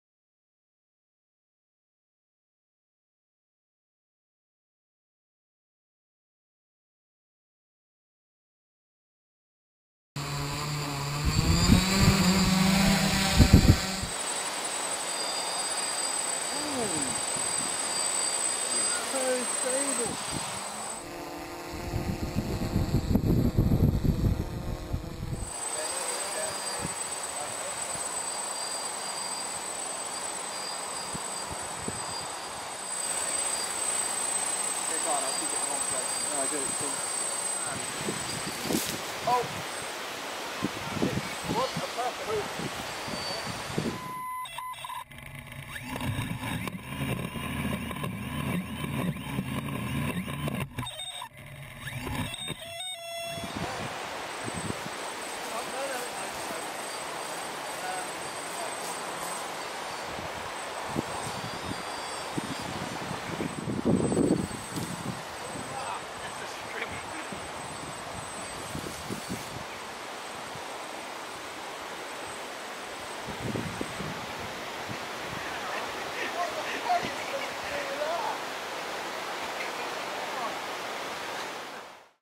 Quad first flight
First flight of my new quadcopter. Very exciting, so easy to fly and so stable, untill the bit where we almost got strimmed.